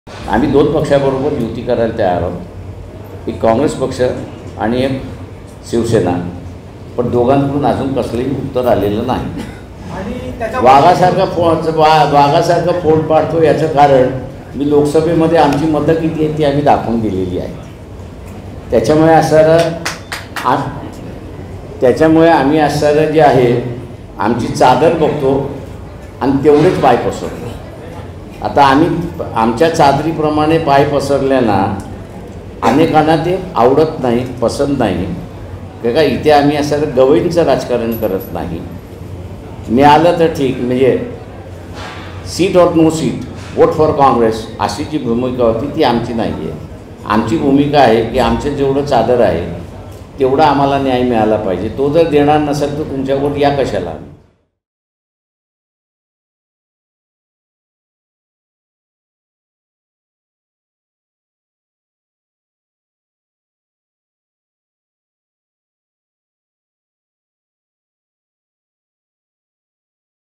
Prakash Ambedkar | 'काँग्रेस-शिवसेनेला युतीचा पर्याय दिलाय' | LetsUpp Marathi
आम्ही काँग्रेस आणि शिवसेनेला युतीसाठी पर्याय दिले आहेत, पण अजून उत्तर आले नसल्याची माहिती वंचित बहुजन आघाडीचे अध्यक्ष प्रकाश आंबेडकर यांनी दिलीय. त्यांनी आज अमरावतीत पत्रकारांशी संवाद साधला.